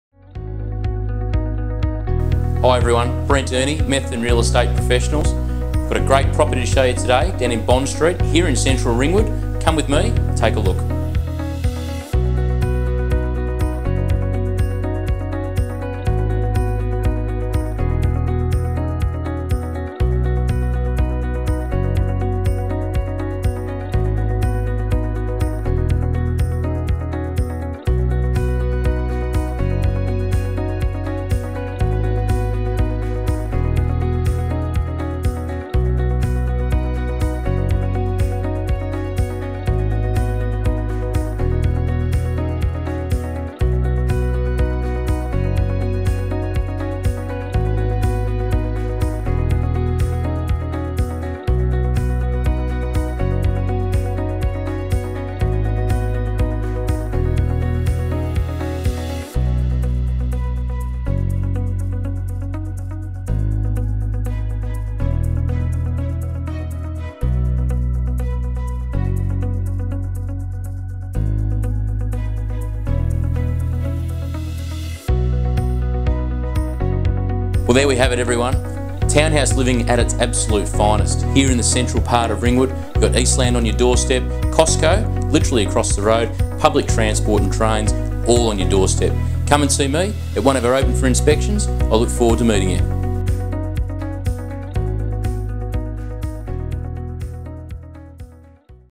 5/12-14 Bond Street, Ringwood
Positioned for convenience, this villa style double storey townhouse is footsteps away from Eastland Shopping Centre, Costco, a vast selection of local eateries and cafes, as well having the train station also within a quick walk. Private, timeless and ready for your enjoyment, featuring 3 bedrooms, comfortable living, dining, a spotless kitchen and a single garage.